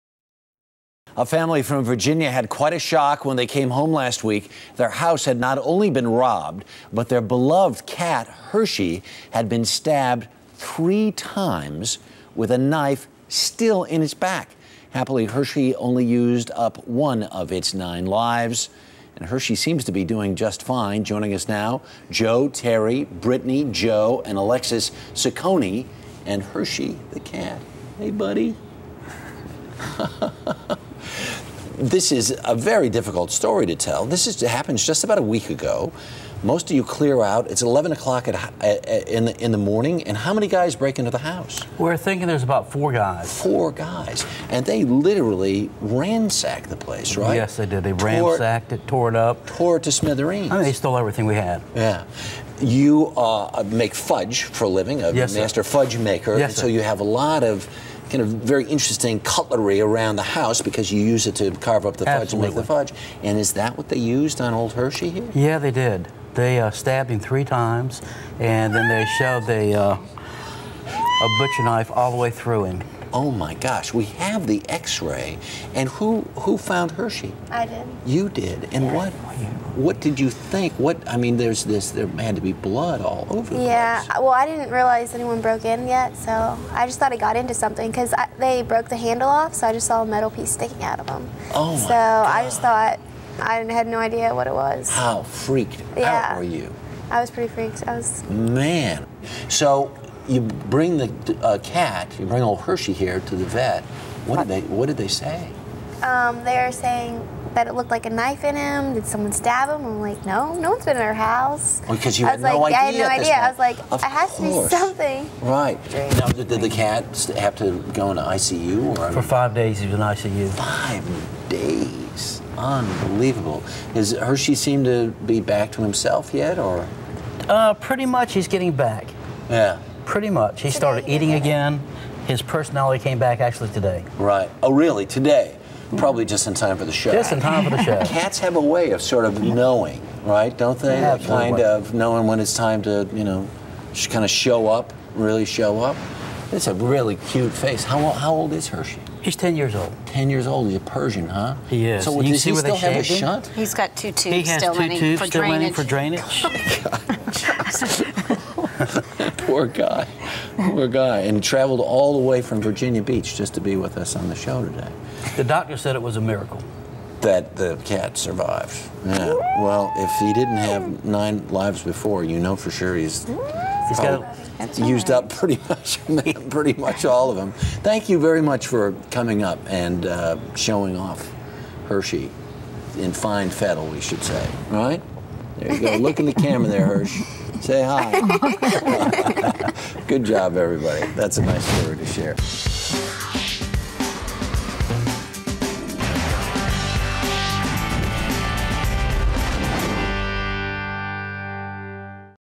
Cat Survives Stabbing
Robbers ransacked a Va. family home and stabbed the family cat three times.  Miraculously, the cat survived the attack and is fully recovered.  The family spoke with Harry Smith.